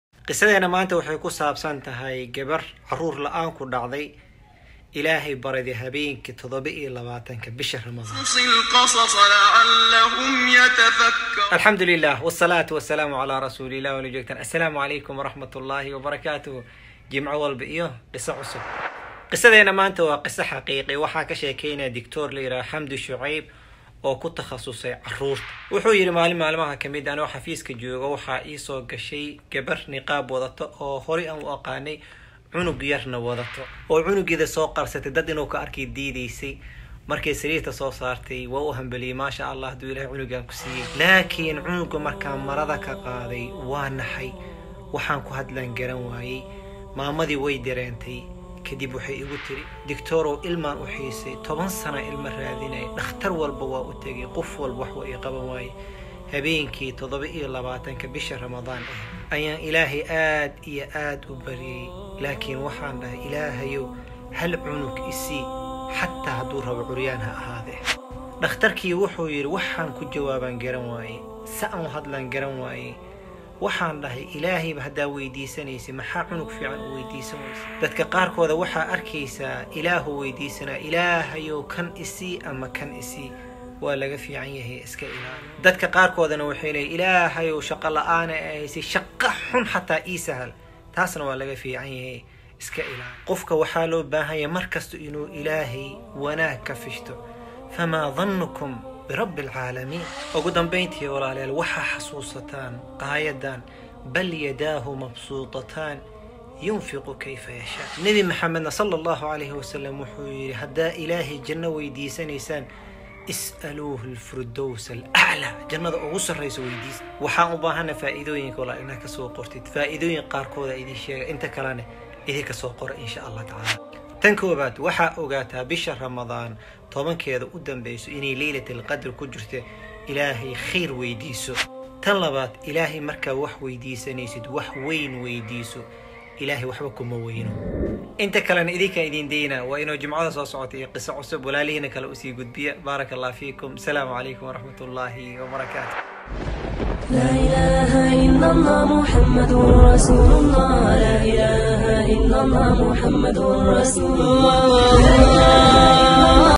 👇👍 JIMCA WALBA IYO QISO CUSUB 👇👍 Waa Gabar Caruur La’aan Ku Dhacday Allah Bariday 27 Ramadan
👇👍👍 JIMCA WALBA IYO QISO CUSUB 👇👍👍
Gabar Caruur La’aan Ku Dhacday.. Allah Bariday Habeenka 27 Ramadan
 👆👆👆Maxaa U Danbeydoono??👆👆👆